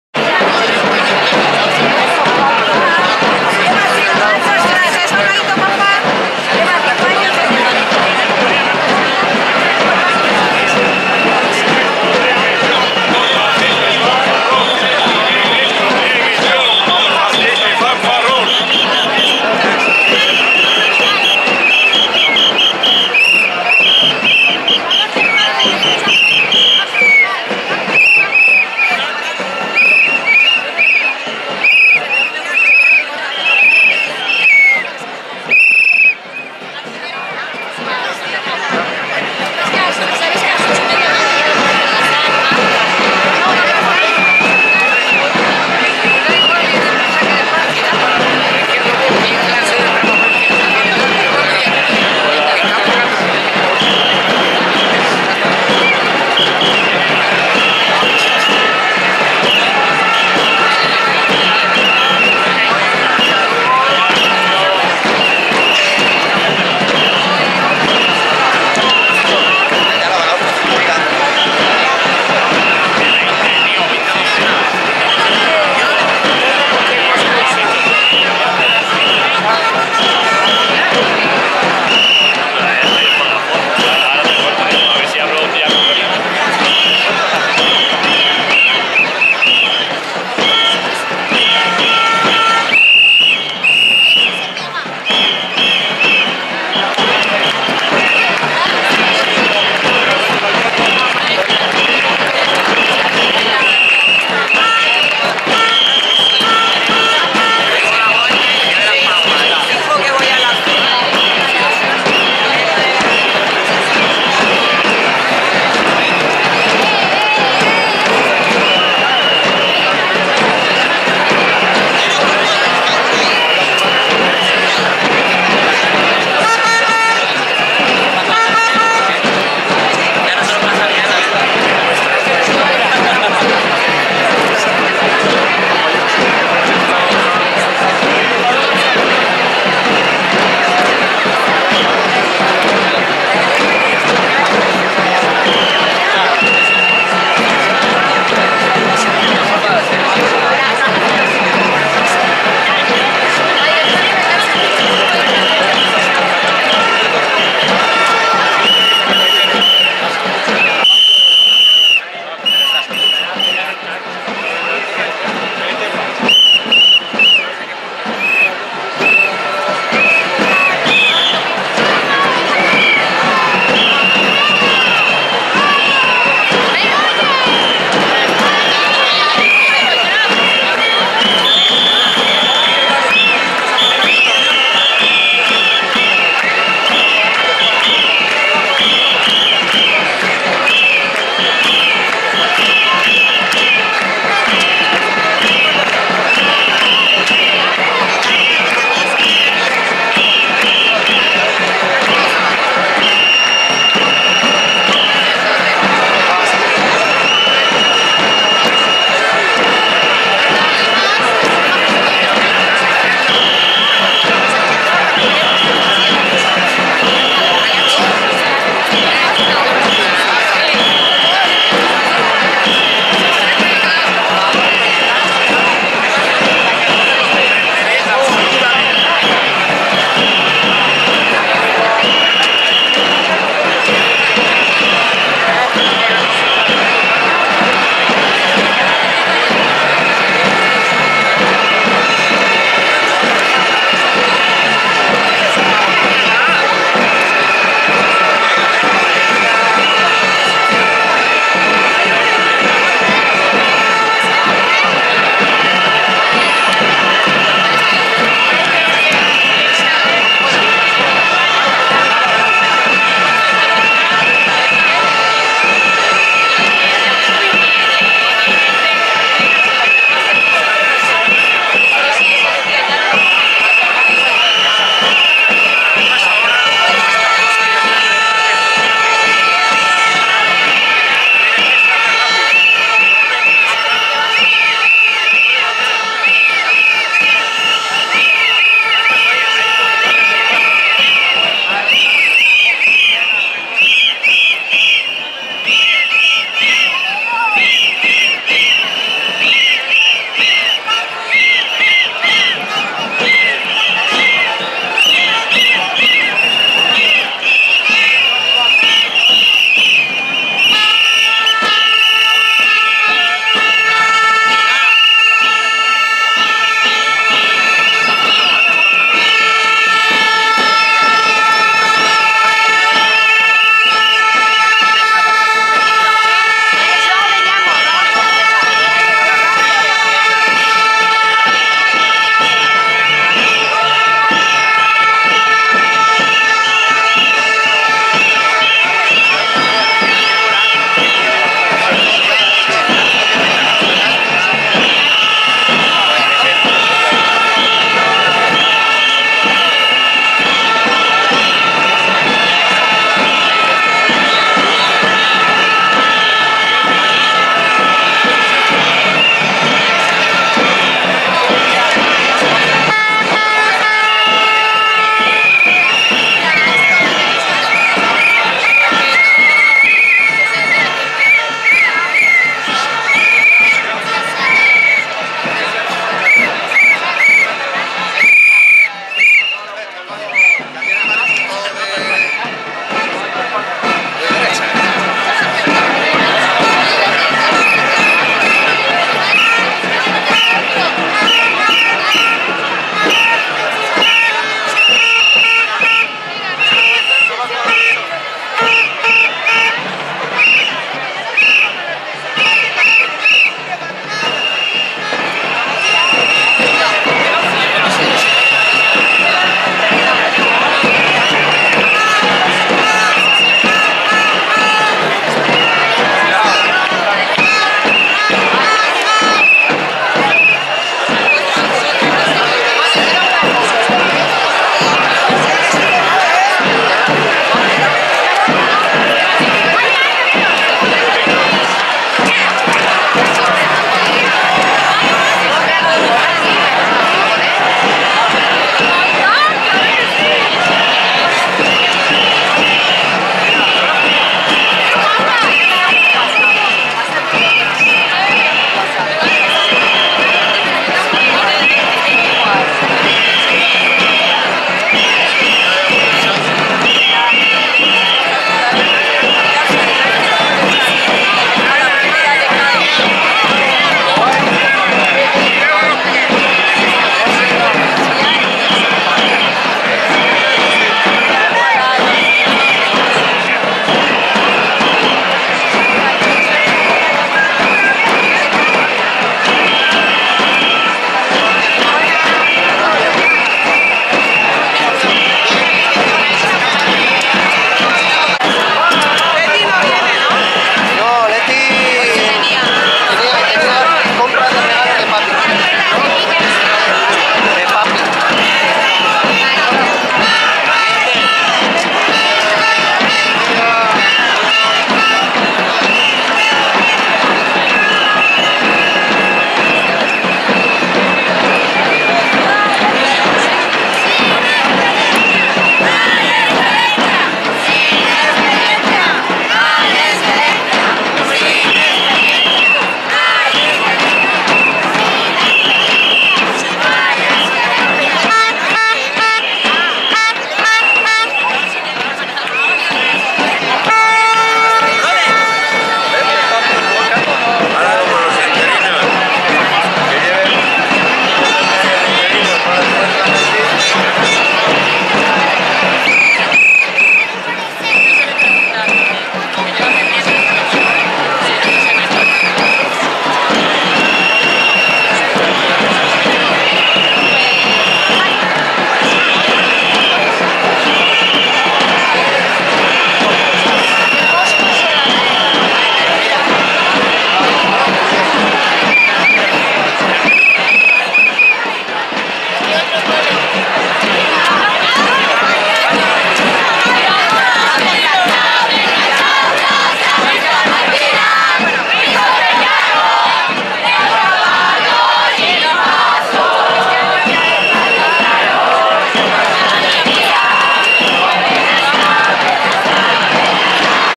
2 Manifestación contra la reforma educativa LOMCE
Manifestación contra la reforma educativa LOMCE, ley Wert
¡No a la contrareforma franquista!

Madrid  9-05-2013